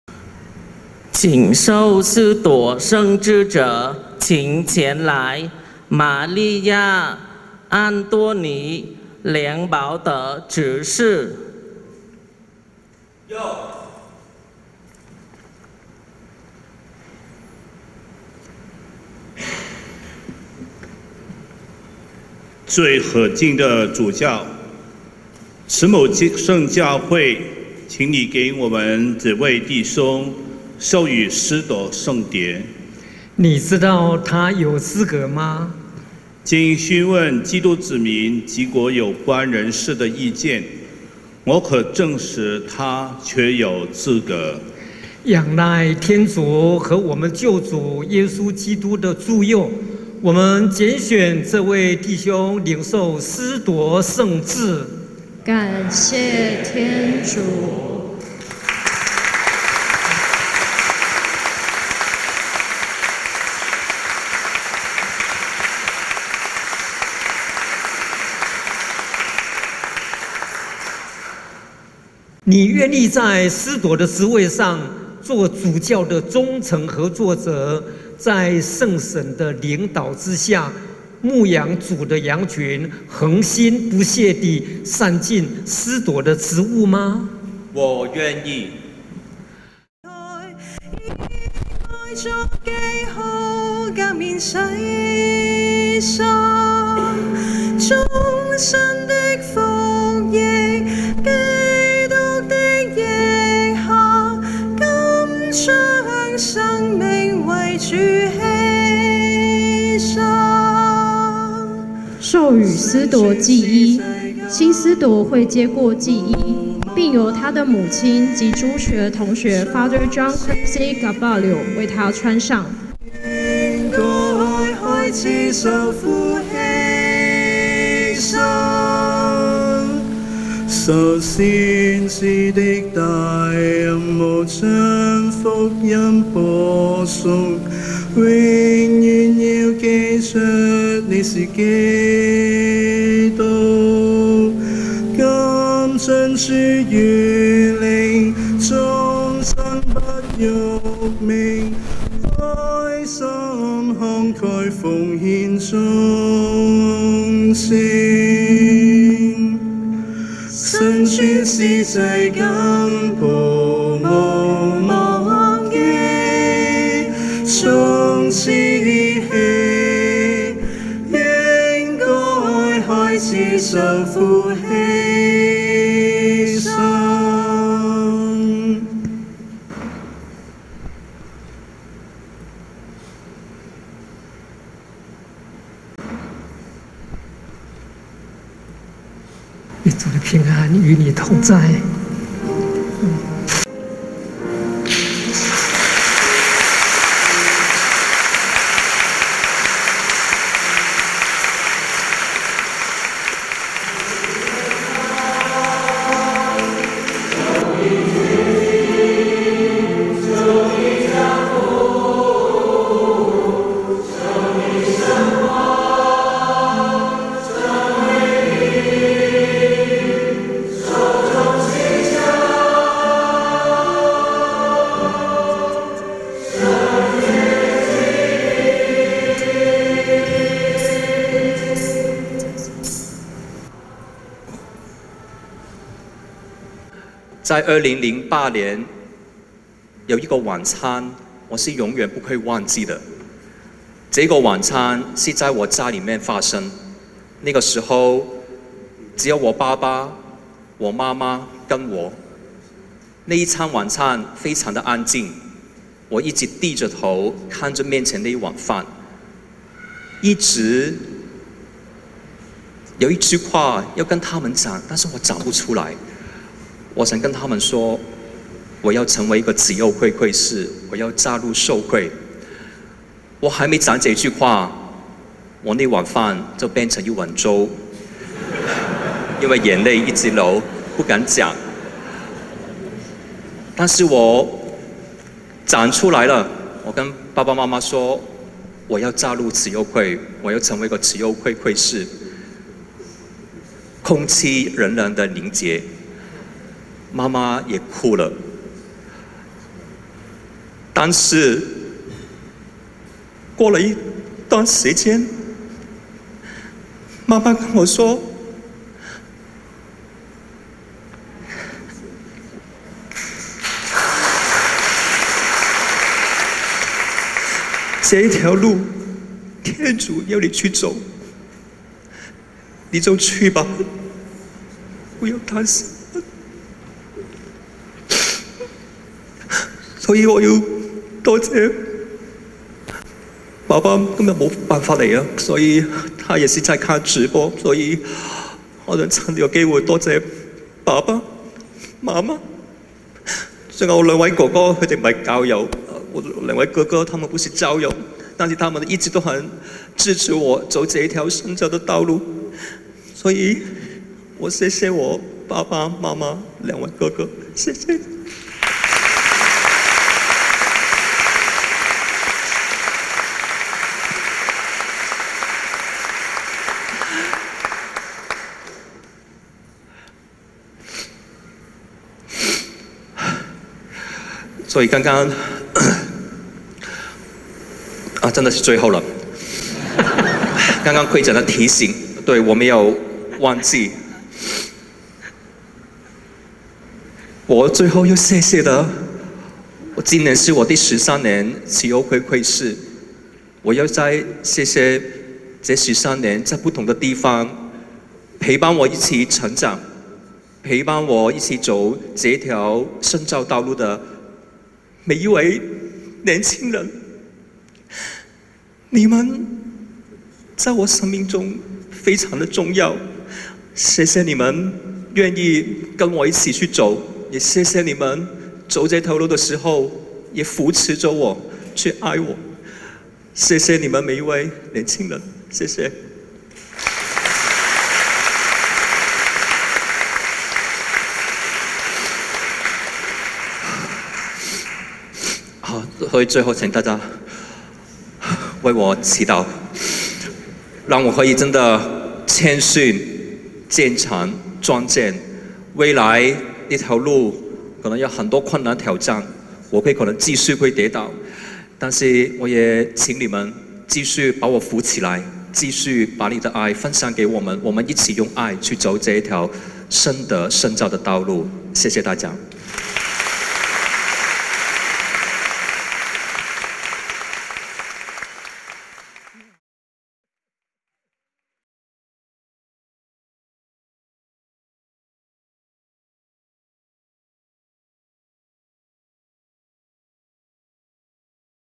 慈幼會梁保得【領受司鐸聖秩】「一碗飯」成為「一碗粥」
在經過慈幼會13年的培育，梁保得執事8月24日在鮑思高堂鍾安住總主教主禮下領受司鐸聖秩，成為新司鐸致詞說，2008年全家用餐的一個晚上忍著向父母道出「加入慈幼會的心願」。